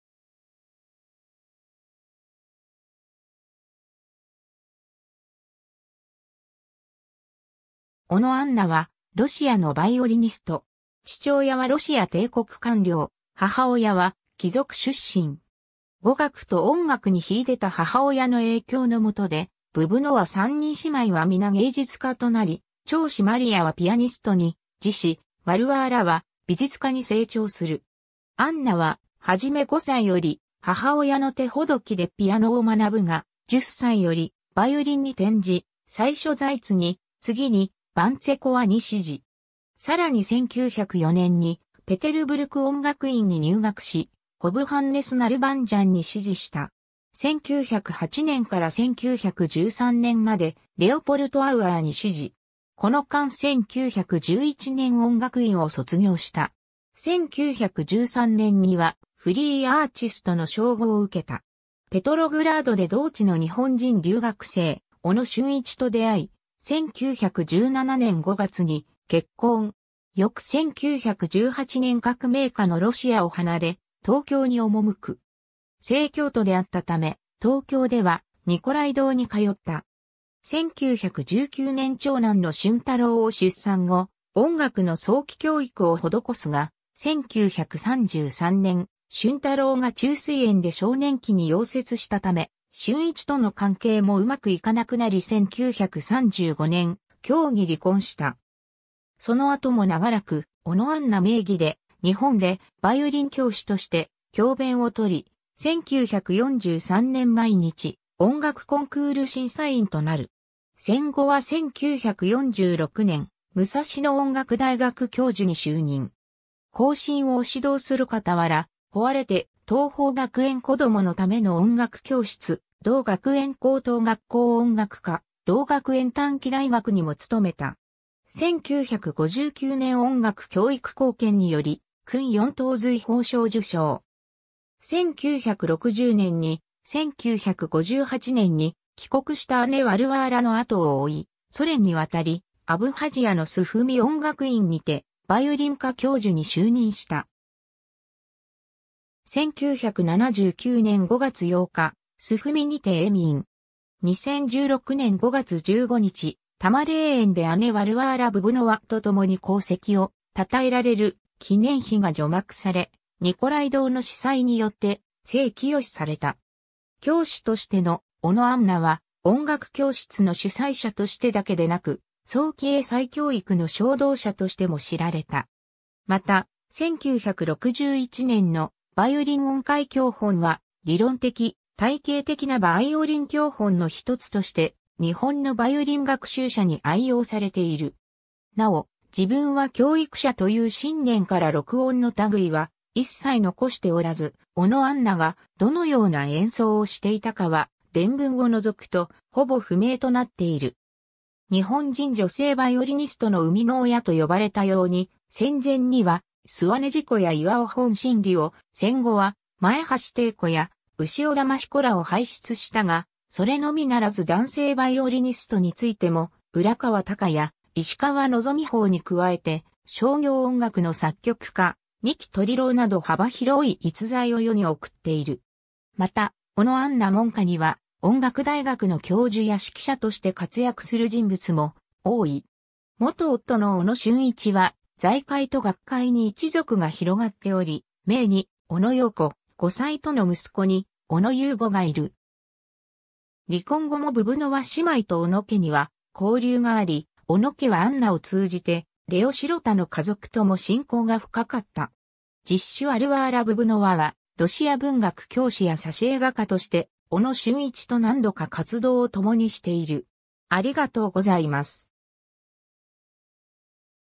小野アンナ
武蔵野音楽大学の教員_(音楽)
小野 アンナ（おの アンナ、本名：アンナ・ディミトリエヴナ・ブブノワ、ロシア語: А́нна Дми́триевна Бубно́ва-О́но, ラテン文字転写: Anna Dmitrievna Bubnova-Ono、1890年3月14日 - 1979年5月8日）は、ロシアのヴァイオリニスト。
 父親はロシア帝国官僚、母親は貴族出身。
語学と音楽に秀でた母親の影響のもとで、ブブノワ3人姉妹はみな芸術家となり、長姉マリヤはピアニストに、次姉ワルワーラは美術家に成長する。
 アンナははじめ5歳より母親の手ほどきでピアノを学ぶが、10歳よりヴァイオリンに転じ、最初ザイツに、次にヴァンツェコワに師事。
さらに1904年にペテルブルク音楽院に入学し、ホヴハンネス・ナルバンジャンに師事した。
1908年から1913年まで、レオポルト・アウアーに師事。
この間1911年音楽院を卒業した。
1913年にはフリーアーチストの称号を受けた。
ペトログラードで同地の日本人留学生・小野俊一（ロシア文学者・生物学者・昆虫学者・社会運動家）と出逢い、1917年5月に結婚。
翌1918年革命下のロシアを離れ、東京に赴く。
正教徒であったため、東京ではニコライ堂に通った。
1919年長男の俊太郎を出産後、音楽の早期教育をほどこすが、1933年俊太郎が虫垂炎で少年期に夭折したため、俊一との関係もうまくいかなくなり1935年協議離婚した。
その後も長らく「小野アンナ」名義で日本でヴァイオリン教師として教鞭を執り、1943年毎日音楽コンクール審査員となる。
戦後は1946年武蔵野音楽大学教授に就任。
後進を指導するかたわら、請われて桐朋学園子どものための音楽教室、同学園高等学校音楽科、同学園短期大学にも務めた。
1959年音楽教育貢献により勲四等瑞宝章受章。
1960年に、1958年に帰国した姉ワルワーラの後を追いソ連に渡り、アブハジアのスフミ音楽院にてヴァイオリン科教授に就任した。
 1979年5月8日、スフミにて永眠。
 2016年5月15日、多磨霊園で姉ワルワーラ・ブブノワと共に功績を称えられる記念碑が除幕され、ニコライ堂の司祭によって成聖された。
 教師としての小野アンナは、音楽教室の主宰者としてだけでなく、早期英才教育の唱導者としても知られた。
また、1961年の『ヴァイオリン音階教本』（音楽之友社刊）は、理論的・体系的なヴァイオリン教本の一つとして、日本のヴァイオリン学習者に愛用されている。
 なお、「自分は教育者」という信念から録音の類は一切残しておらず、小野アンナがどのような演奏をしていたかは伝聞を除くとほぼ不明となっている。
 日本人女性ヴァイオリニストの生みの親と呼ばれたように、戦前には諏訪根自子や巖本真理を、戦後は前橋汀子や潮田益子らを輩出したが、それのみならず男性ヴァイオリニストについても浦川宜也、石川希峰に加えて、商業音楽の作曲家三木鶏郎など幅広い逸材を世に送っている。
また、小野アンナ門下には、音楽大学の教授や指揮者として活躍する人物も多い。
 元夫の小野俊一は、財界と学界に一族が広がっており、姪にオノ・ヨーコ、後妻との息子に小野有五（北海道大学教授）がいる。
離婚後もブブノワ姉妹と小野家には交流があり、小野家はアンナを通じてレオ・シロタの家族とも親交が深かった。
実姉ワルワーラ・ブブノワは、ロシア文学教師や挿絵画家として、小野俊一と何度か活動をともにしている。